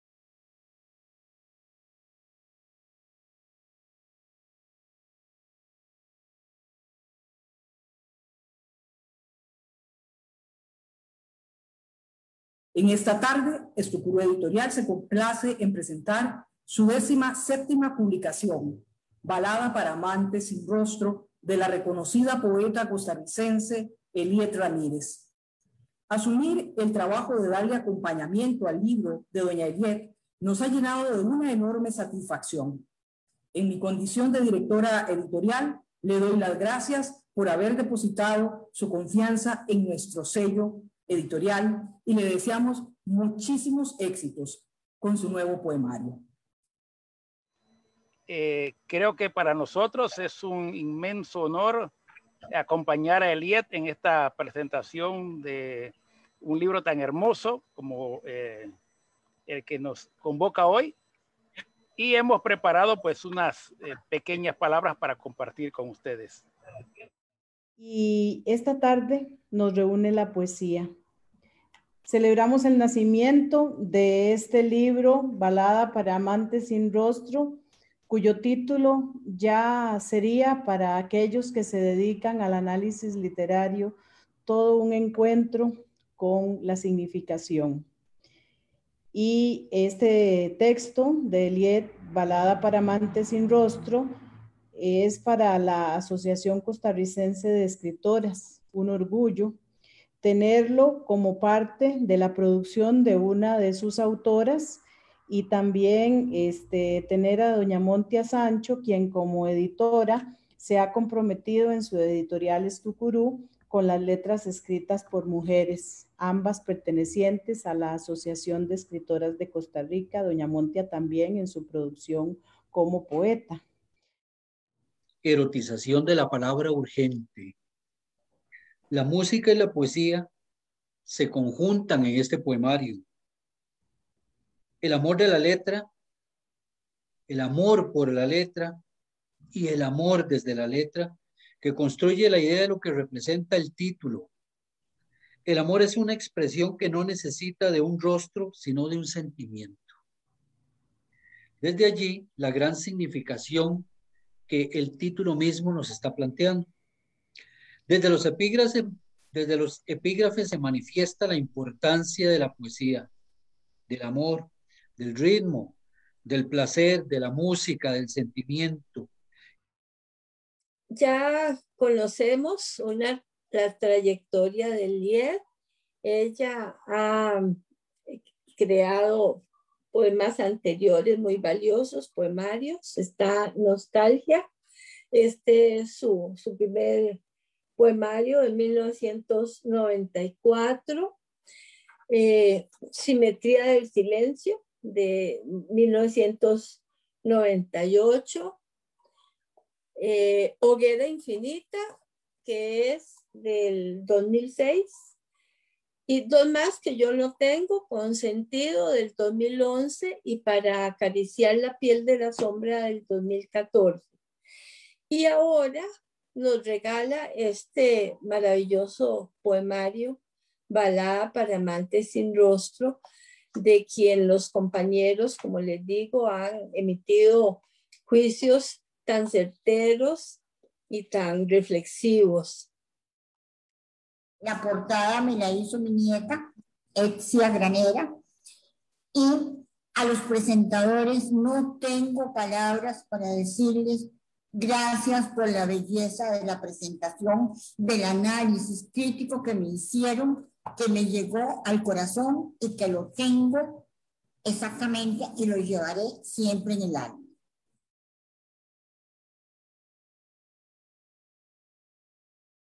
Presentación del libro Balada para amantes sin rostro
La Biblioteca Nacional del Sistema Nacional de Bibliotecas se complace en compartir la presentación del libro "Balada para amantes sin rostro" de Elliette Ramírez, junto con la participación de Peggy von Mayer, Leda Cavallini, Manuel Montilla y Oscar Alvarado como presentadores.